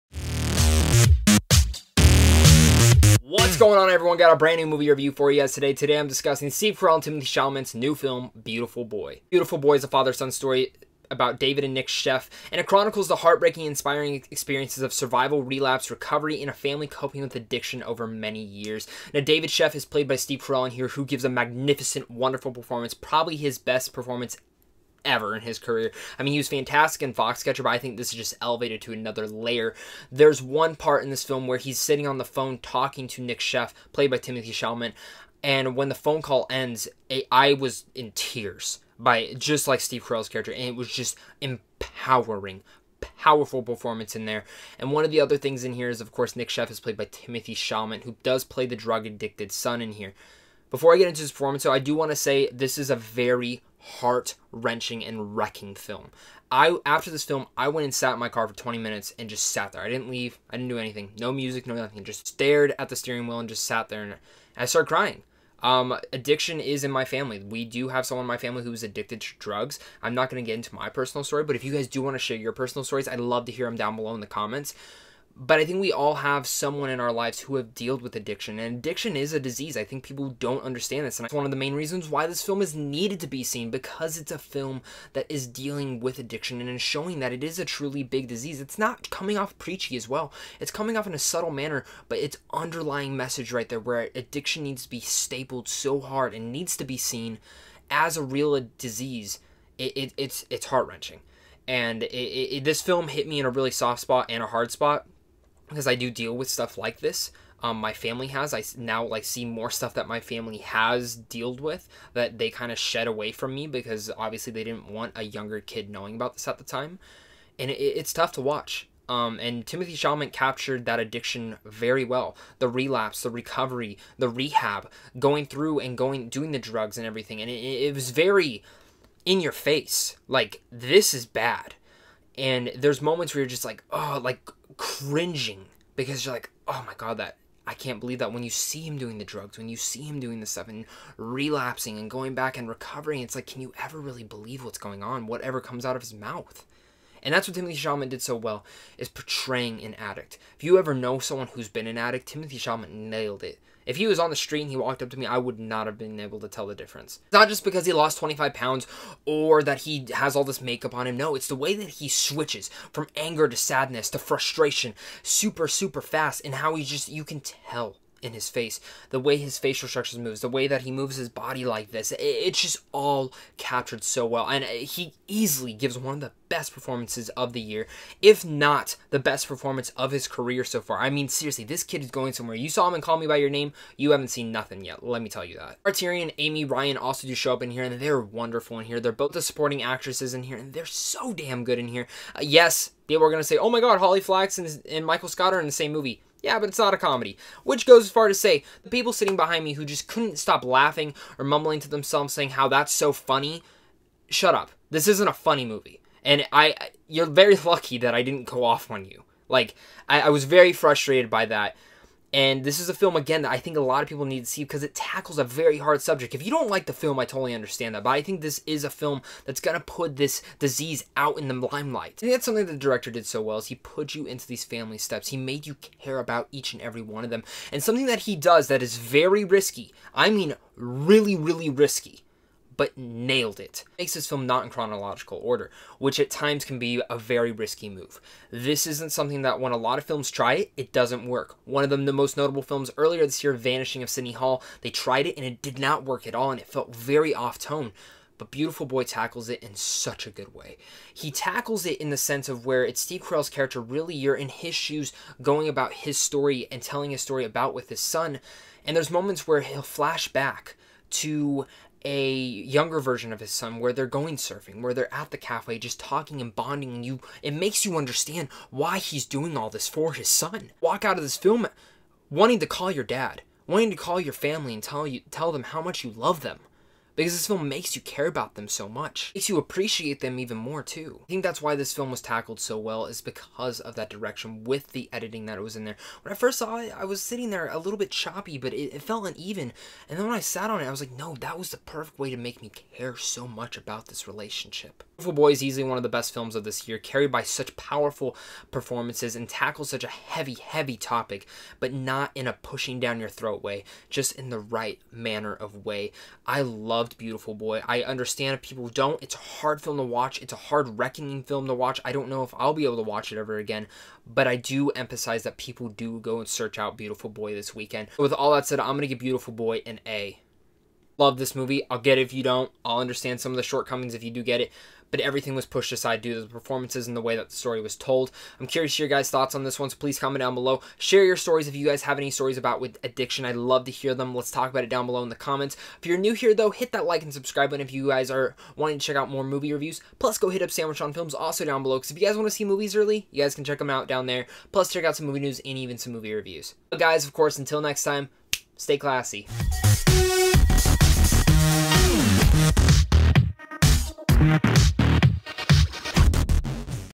BEAUTIFUL BOY (2018) - Movie Review (Timothee Chalamet and Steve Carell movie)
Synopsis:Teenager Nicolas Sheff seems to have it all -- good grades, editor of the school newspaper, actor, artist and athlete. When Nic's addiction to meth threatens to destroy him, his desperate father does whatever he can to save his son and his family.

Cast and Crew:
Directed by Felix Van Groeningen
Produced by
Dede Gardner
Jeremy Kleiner
Brad Pitt
Screenplay by
Luke Davies
Felix Van Groeningen
Based on
Beautiful Boy: A Father's Journey Through His Son's Addiction
by David Sheff
Tweak: Growing Up on Methamphetamines
by Nic Sheff
Starring
Steve Carell
Timothée Chalamet
Maura Tierney
Amy Ryan
Cinematography Ruben Impens
Edited by Nico Leunen
Production
companies
Plan B Entertainm